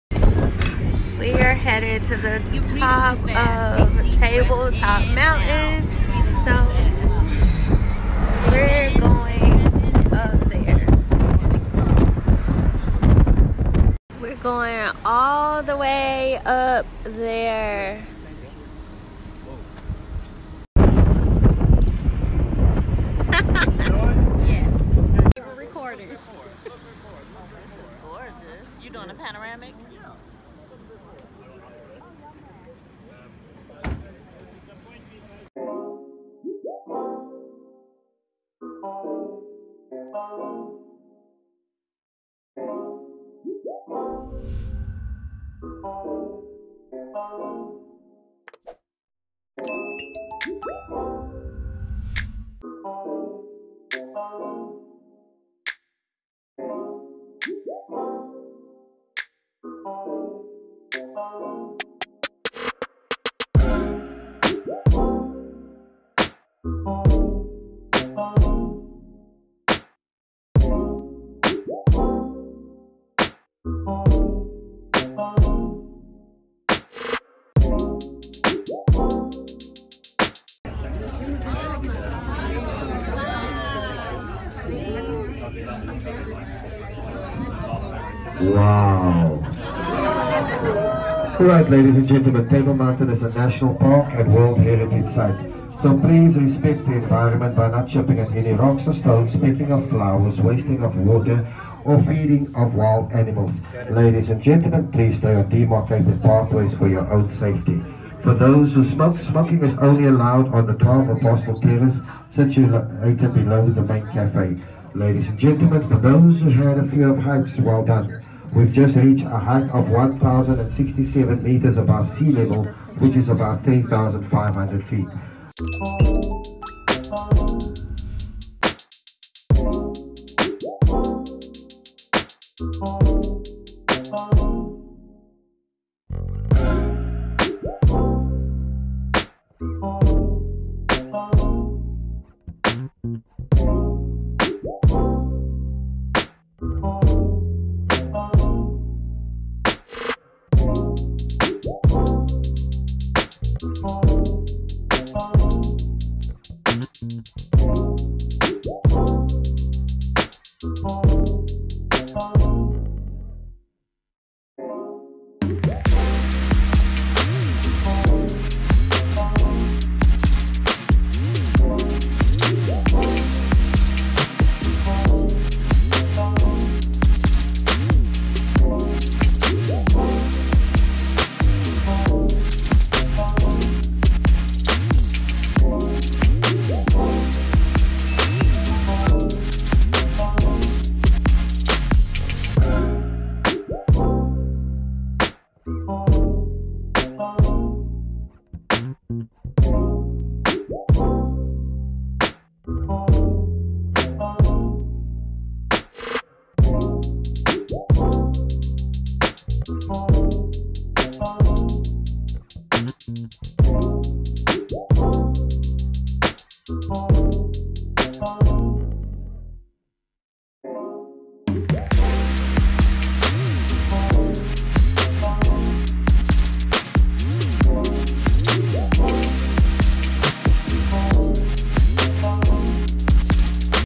Table Mountain National Park Cape Town Travel Vlog | Ebony Meets World
Welcome to another Cape Town Travel Vlog in this travel vlog my friends and I continue our South Africa travel making a stop at one of the 7 natural wonders of the world Table Mountain in Cape Town South Africa.